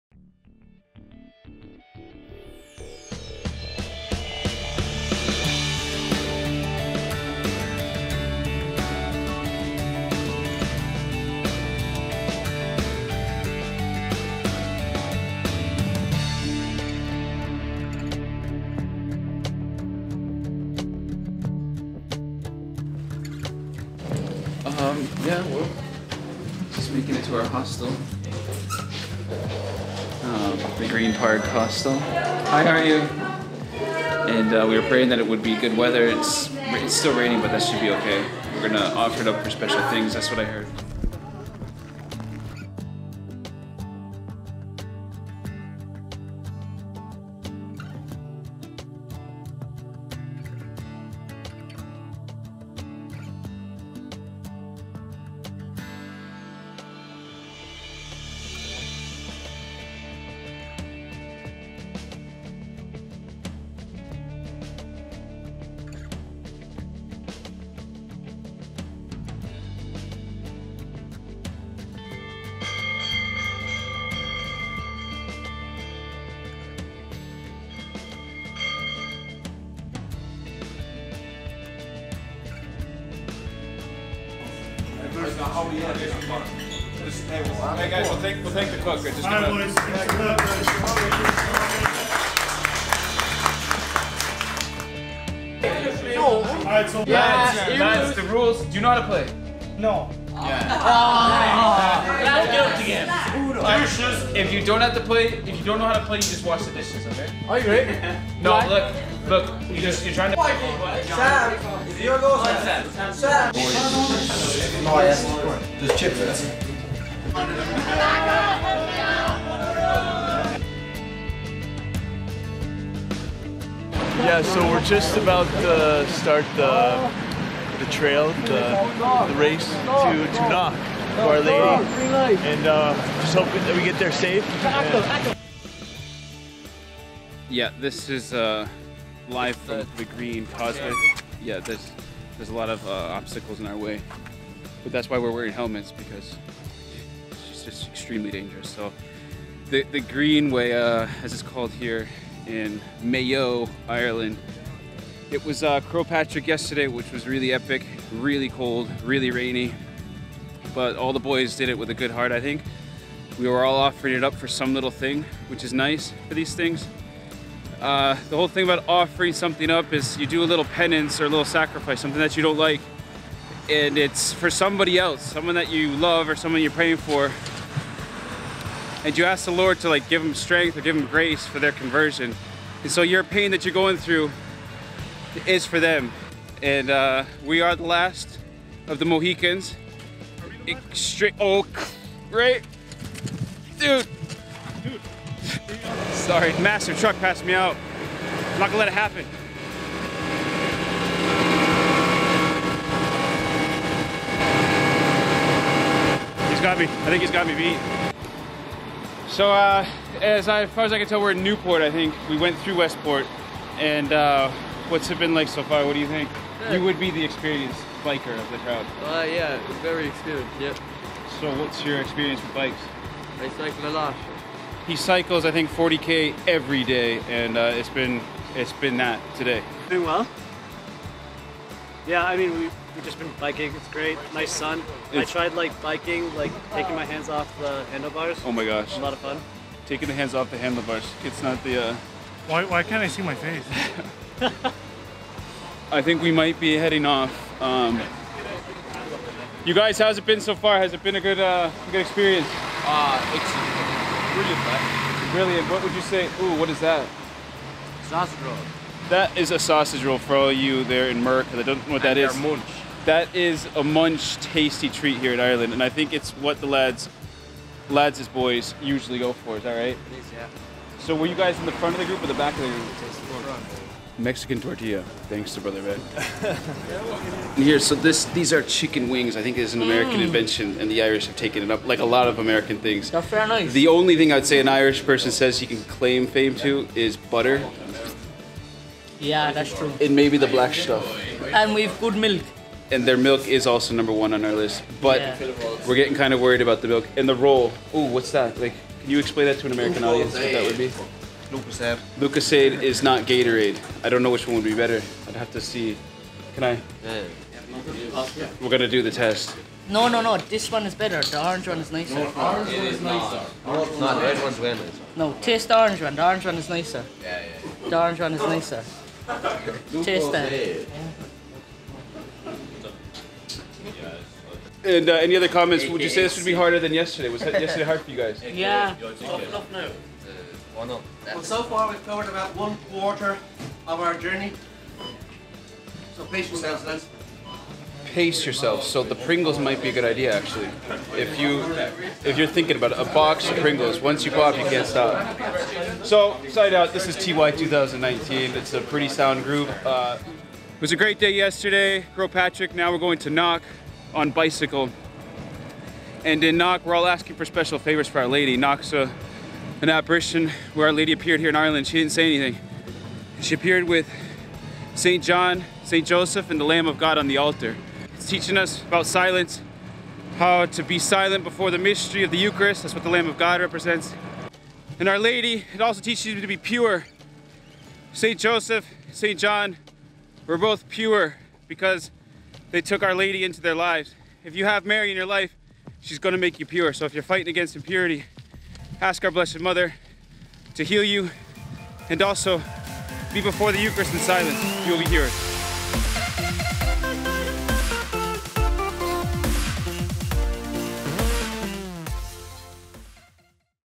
CBS Charleville TY TRIP 2019
Our CBS Charleville pilgrimage to Croagh Patrick and Knock!

This was our amazing pilgrimage with the TY (transition year) up to St. Patrick's Holy Mountain, our cycling path of the Greenway, Co. Mayo, and our visit to Our Lady of Knock.  Are sacrifice, silence, and purity important? What do you think?